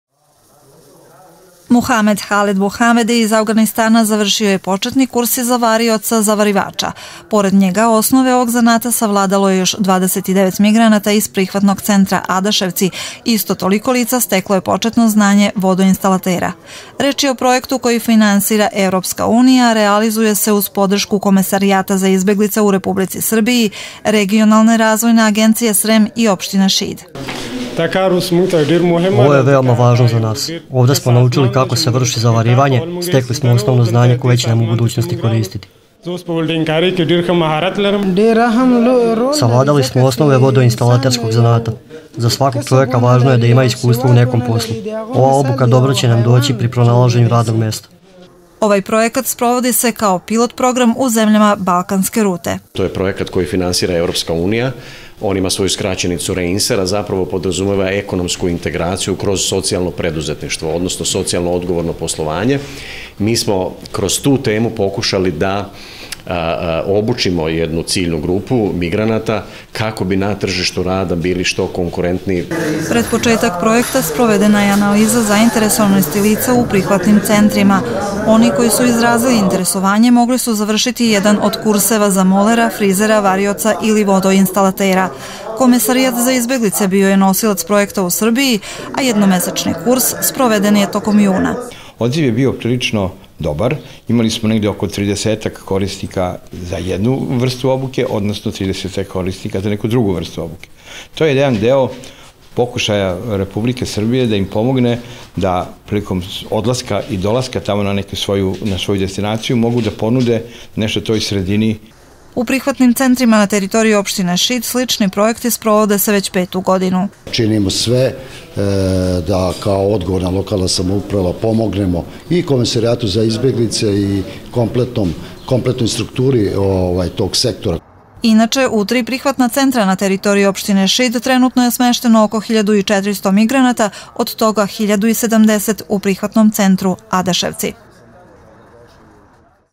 Sertifikati za migrante
Migrantima u Prihvatnom centru u Adaševcima danas su podeljeni sertifikati o završenom početnom kursu za varioce-zavarivače i vodoinstalatere. Kurs je realizovan u sklopu projekta Ekonomske integracije izbeglica korz socijalno preduzetništvo, a obučeno je oko 60 lica